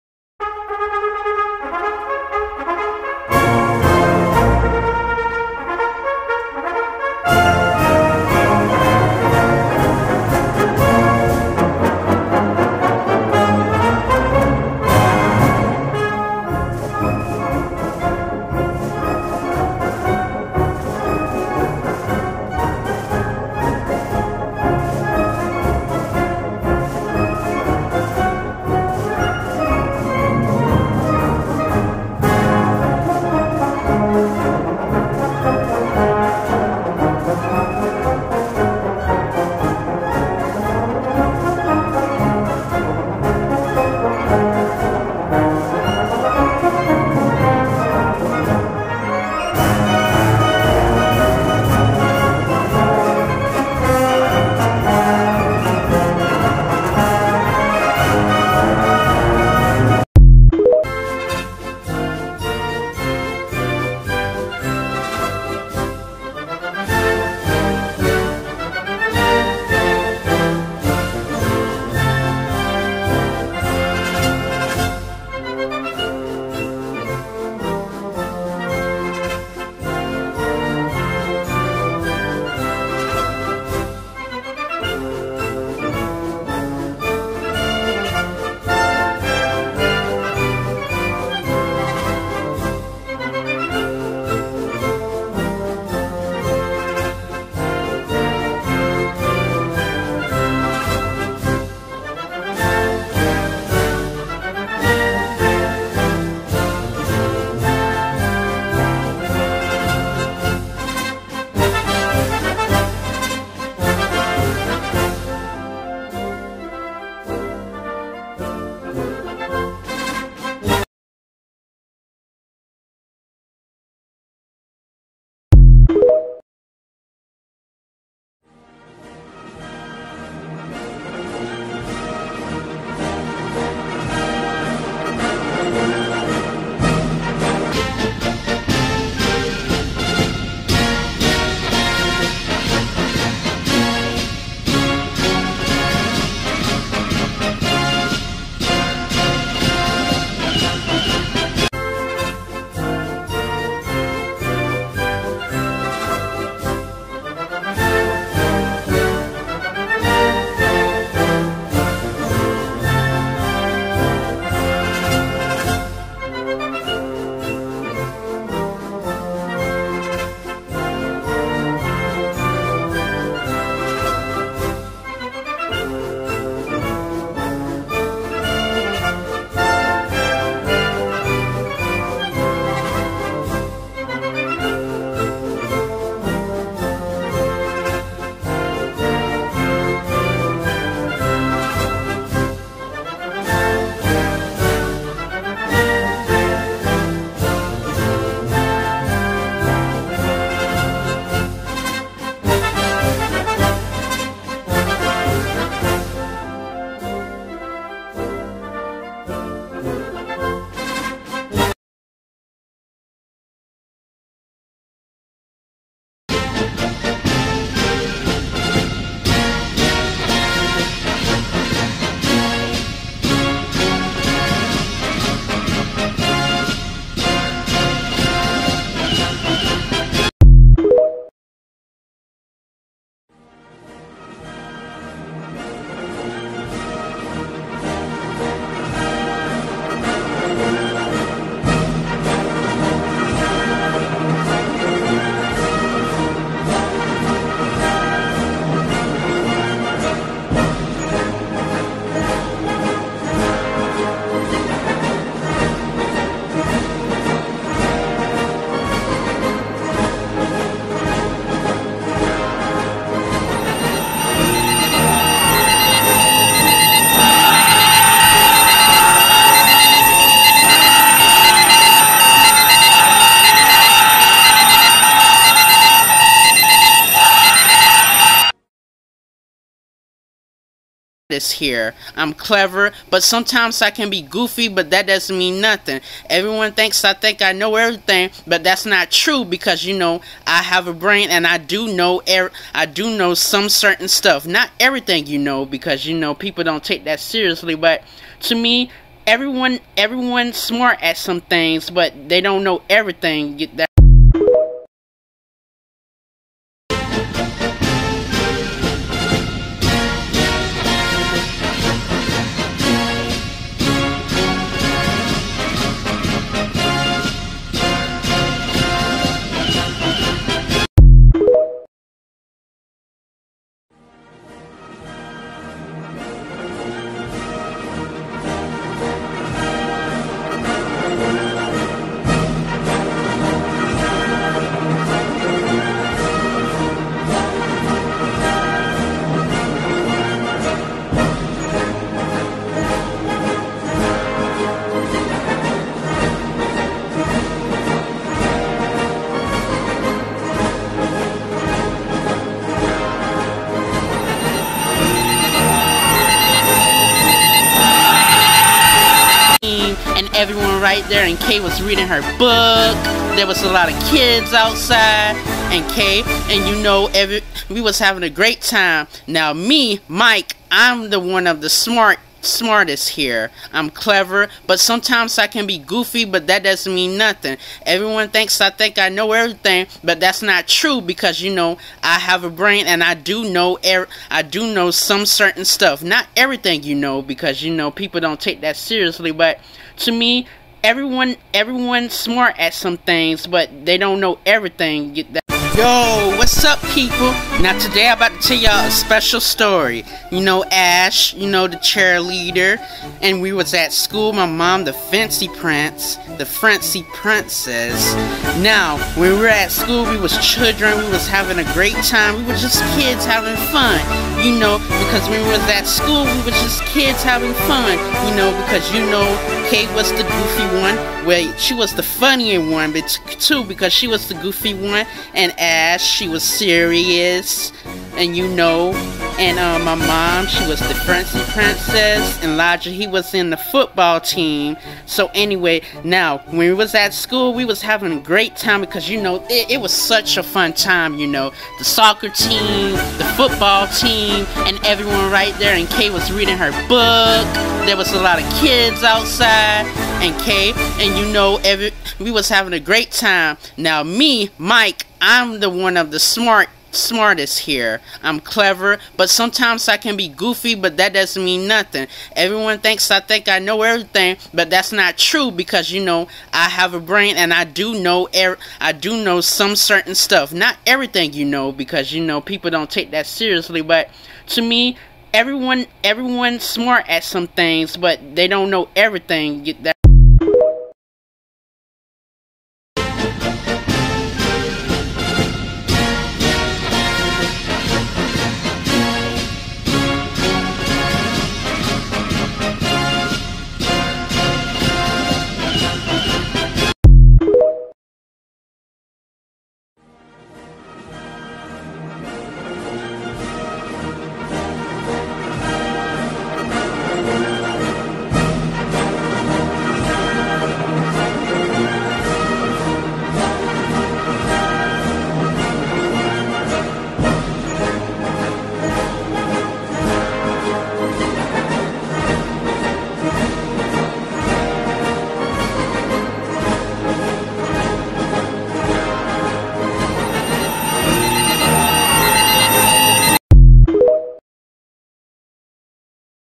Mike's Fantastic Adventures The Animated Movie
Watch Mike's First Ever Real Movie Mike Tells his story about his life The Mike Cinematic Universe gets Bigger.